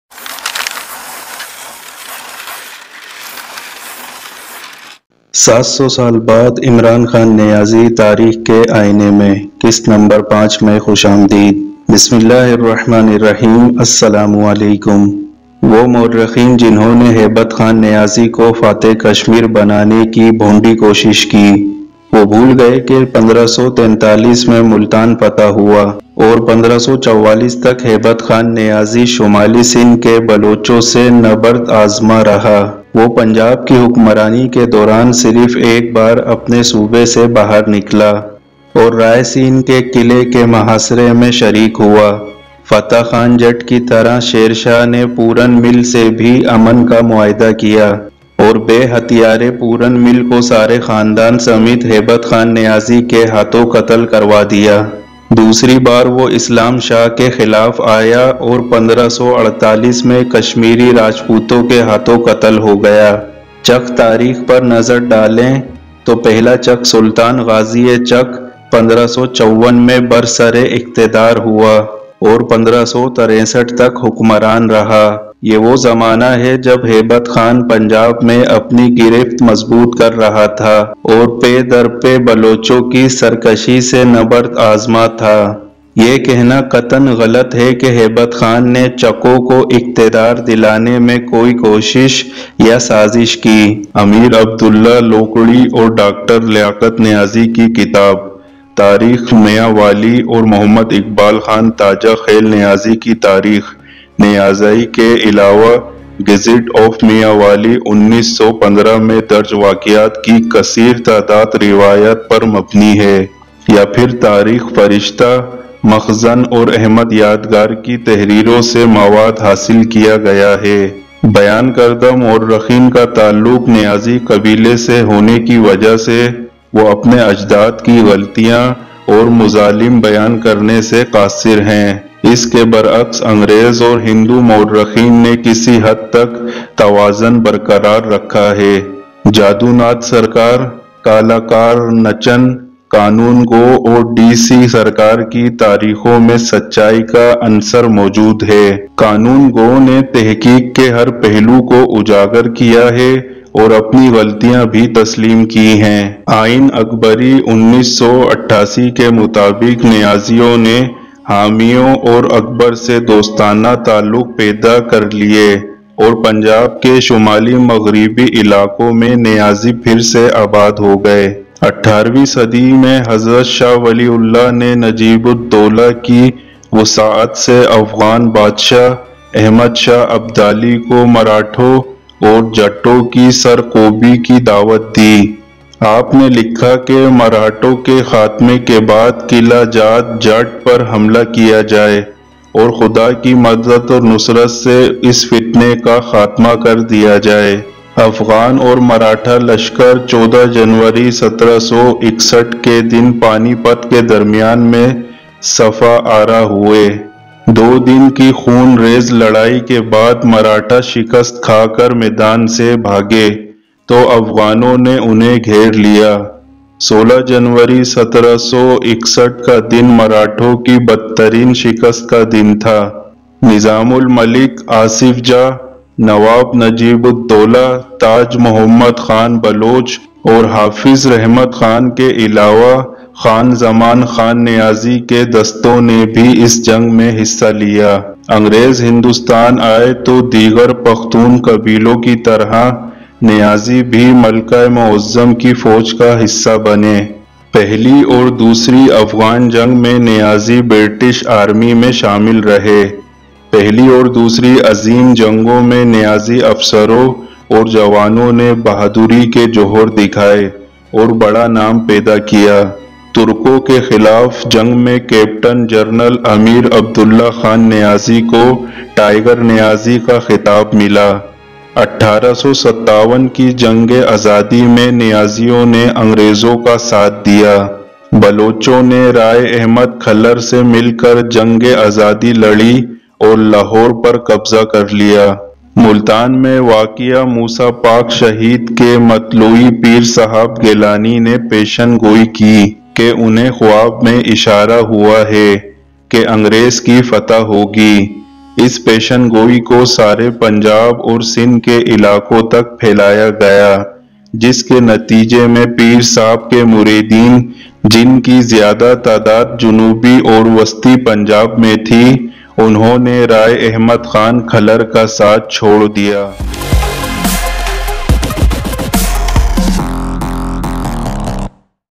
History of Niazi | History of Niazi Pathan | EPISODE - 05 | Urdu & Hindi
Episode No. 5 - Seven Hundred Years Later (Imran Khan Niazi In the mirror of history,

Those historians who tried to make Haibat Khan Niazi the conqueror of Kashmir forgot that Multan was conquered in 1543 and till 1544 Haibat Khan Niazi was fighting against the Balochs of Northern Sindh. He ventured out of his province only once during his rule of Punjab and took part in the siege of Rai Sen's fort. Like Fateh Khan Jat, Sher Shah also made a peace treaty with Puranmal, and the unarmed Puranmal was terrorized with his entire family. He was killed by Khan Niazi. The second time he came against Islam Shah and was killed by the Kashmiri Rajputs in 1548.
Look at Chak History The first Chak Sultan Ghazi Chak came to power in 1554 and ruled till 1563. This was the time when Haibat Khan was consolidating his hold on Punjab and was struggling with successive Baloch rebellions.
It is absolutely wrong to say that Haibat Khan made any attempt or conspiracy to bring power to the Chakas. Amir Abdullah Rukdi and Dr. Liaqat Niazi's book Tarikh Mianwali and Muhammad Iqbal Khan Tajakhel Niazi's Tarikh Niazi, besides the Gazette of Mianwali 1915, a large number of incidents are based on traditions or Tarikh Farishta, Makhzan and Ahmad Yadgar's writings. The material has been obtained. As the mentioned historians belong to the Niazi tribe, they are unable to narrate the mistakes and atrocities of their forefathers.
On the contrary, British and Hindu historians maintained some balance. There is an element of truth in the histories of Ja Donath Sarkar, Ka Lakaranchan Law Go and DC Sarkar. Law Gone has highlighted every aspect of the research and has also admitted his mistakes. According to the Akbari constitution of 1588, the Niazis established friendly relations with Humayun and Akbar and the Niazis settled again in the northwestern regions of Punjab.
In the 18th century, Hazrat Shah Waliullah invited the Afghan king Ahmed Shah Abdali to suppress the Marathas and Jats through the mediation of Najib-ud-Daula. This fitna should be ended. The Afghan and Maratha armies lined up on the field of Panipat on 14 January 1761.
After two days of bloody fighting, the Marathas fled the field after being defeated, and the Afghans surrounded them. 16 January 1761 was the worst defeat of the Marathas. Apart from Nizam-ul-Mulk Asif Jah, Nawab Najib-ud-Daula, Taj Muhammad Khan Baloch and Hafiz Rahmat Khan, the troops of Khan Zaman Khan Niazi also participated in this battle. When the British came to India, like other Pakhtun tribes, the Niazis also became part of the army of Queen Muazzam.
Niazi joined the British Army in the first and second Afghan wars. In the First and Second Great Wars, Niazi officers and men showed the essence of bravery and made a great name. In the war against the Turks, Captain (General) Amir Abdullah Khan Niazi got the title of Tiger Niazi. In the War of Independence in 1857, the Niazis sided with the British. The Baloch fought the War of Independence together with Rai Ahmad Kharl and captured Lahore.
Pir Saheb Geelani, the guardian of Musa Pak Shaheed in Multan prophesied that he had been told in a dream that the British would win. This prophecy was spread to the entire Punjab and Sindh regions, as a result of which Pir Sahib's followers, who were mostly in southern and central Punjab, left the side of Rai Ahmad Khan Kharal.